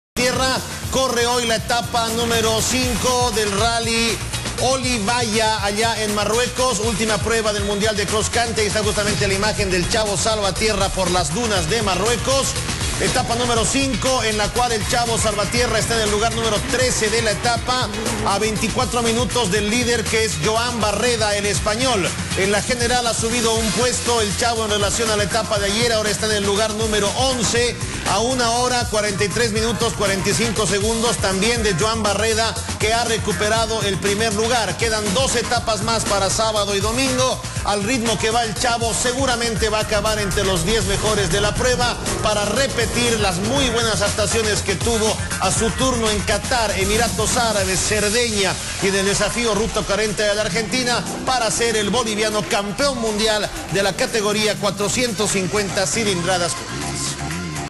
Rally de Marruecos
"Chavo" Salvatierra va undécimo en la competencia.